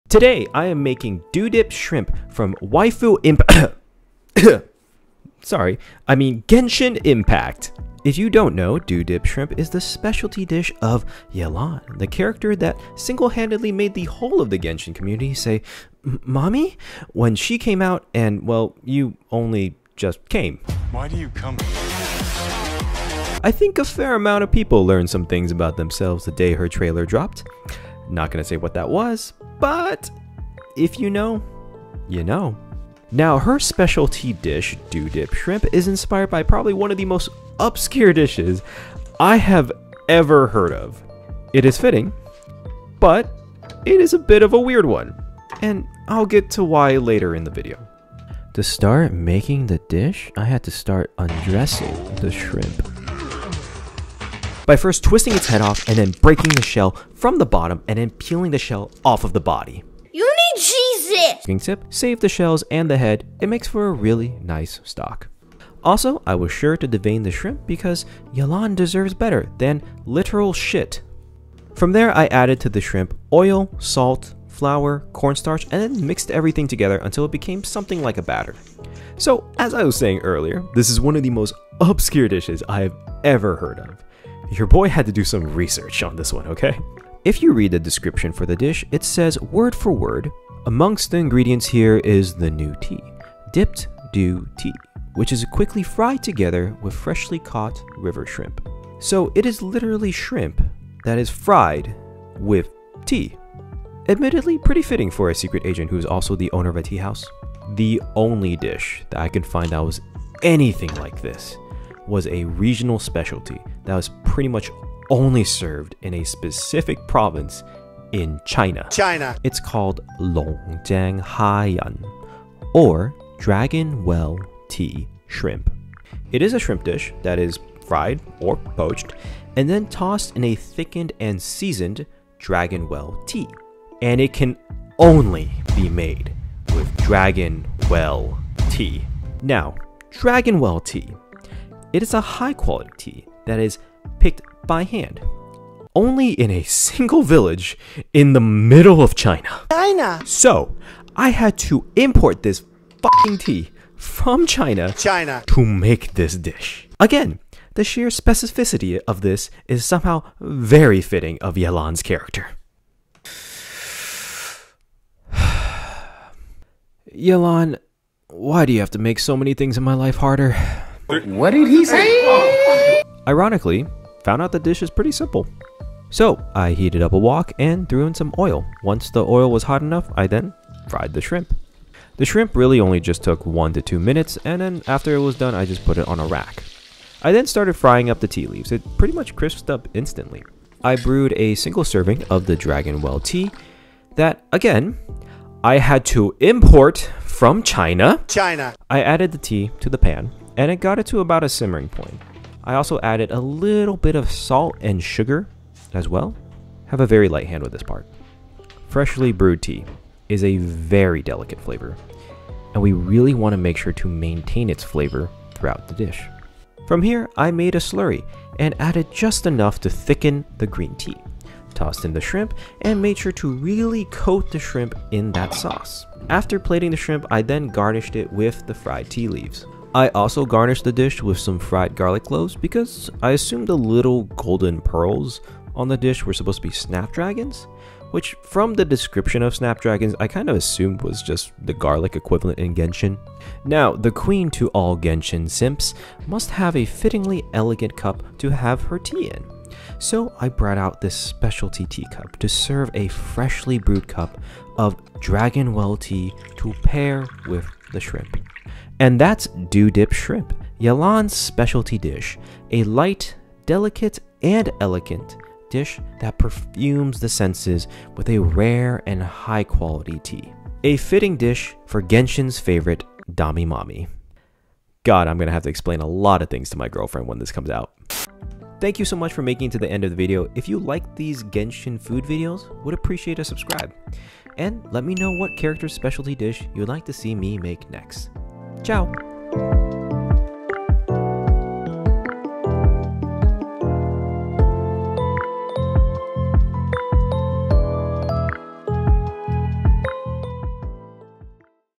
Genshin Impact Food | Yelan's "Dew Dipped Shrimp" IRL
Dew Dipped Shrimp IRL version.

Let me know what Genshin Impact character's speciality dish I should make next!

JOIN GAIN EXP FITNESS TODAY!
Gain EXP Fitness YouTube ►    / @gainexpfitnessof...